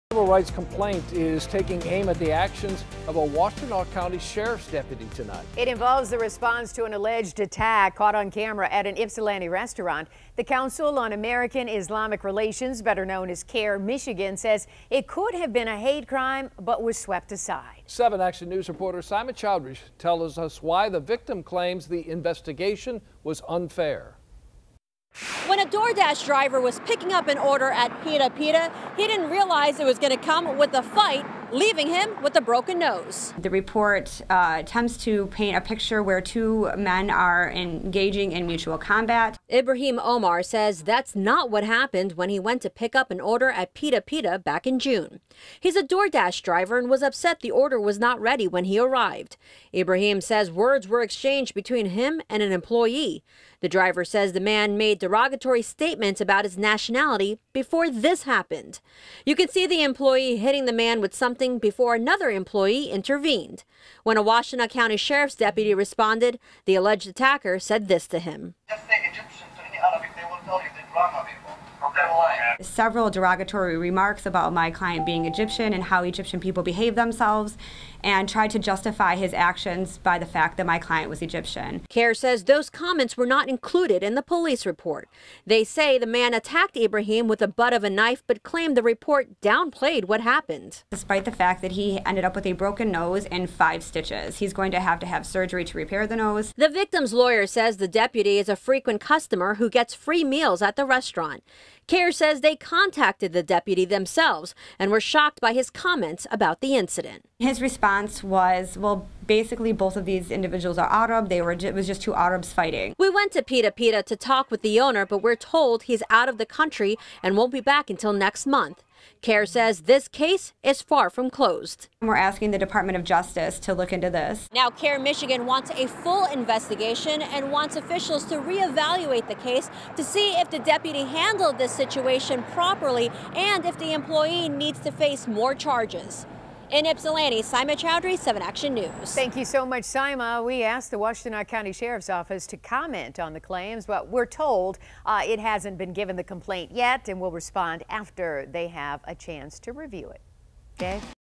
Council on American-Islamic Relations filed complaint over handling of alleged restaurant assault
Did a Washtenaw County Sheriff’s Deputy act fairly when responding to call about an attack at a Ypsilanti restaurant?  That’s what CAIR-Michigan is asking after filing a complaint.  They claim the deputy mishandled an assault case because he is a frequent customer and knows the owner.